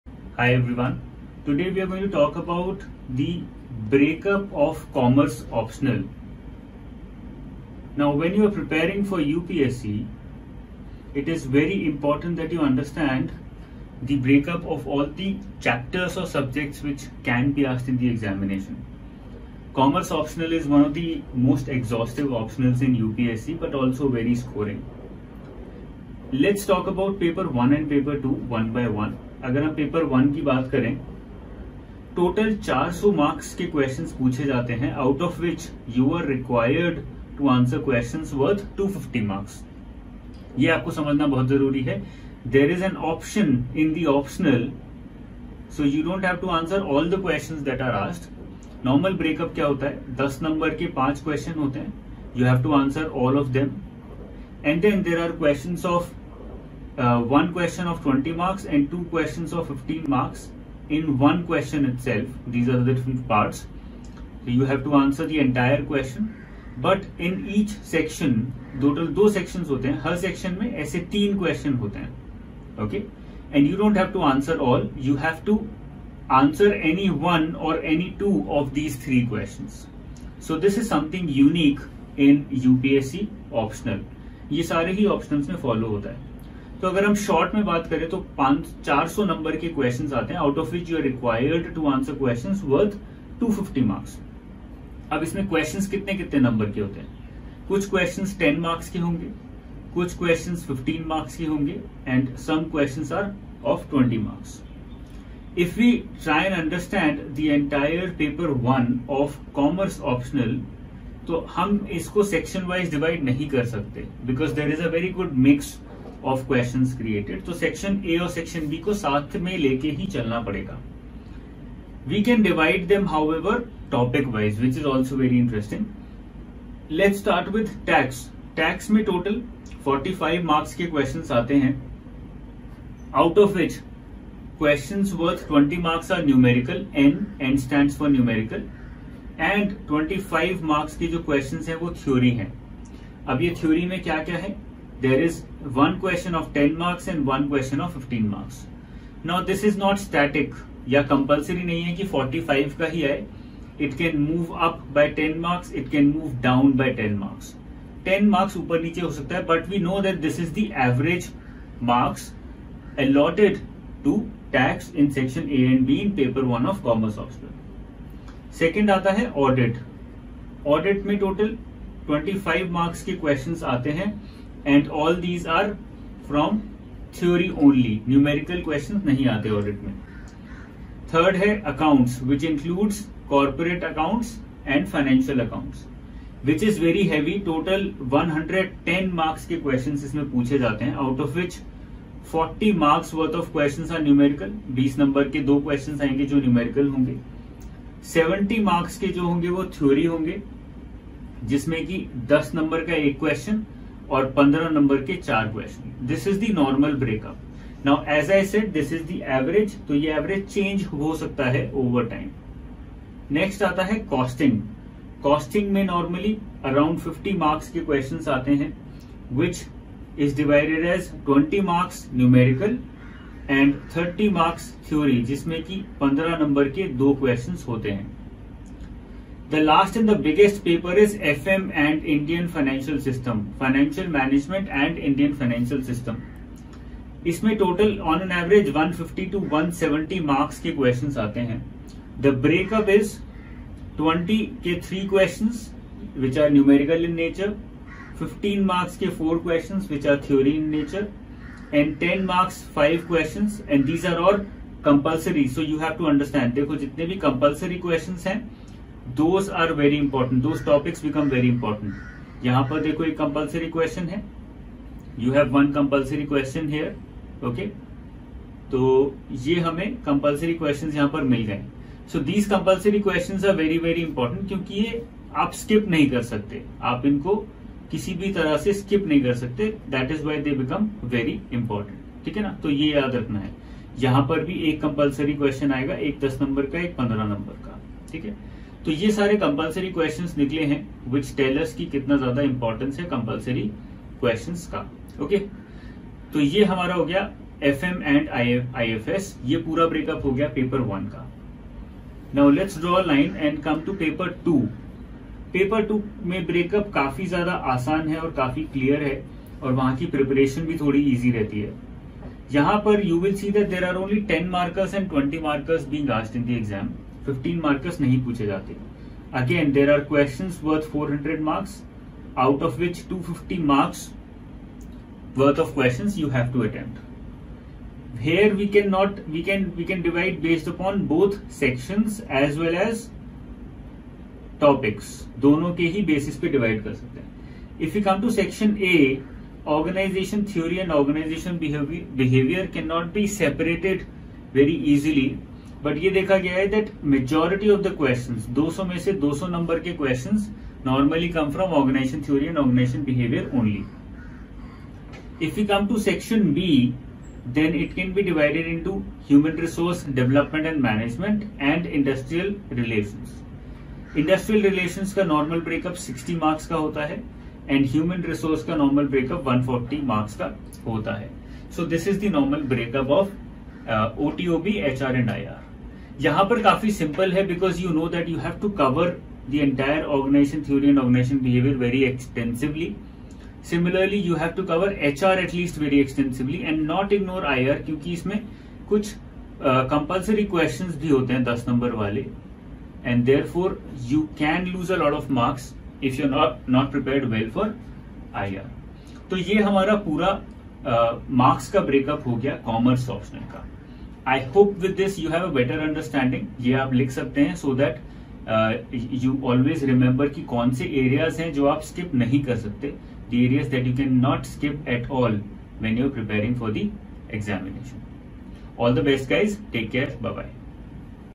UPSC Commerce | Marks Breakup of commerce optional | UPSC commerce optional analysis|Anuj Jindal sir
All of us want to clear upsc with the best scores but the first step towards this target is to understand from where are the questions asked! There’s always a pattern. Don’t worry, I have de-coded the pattern for you here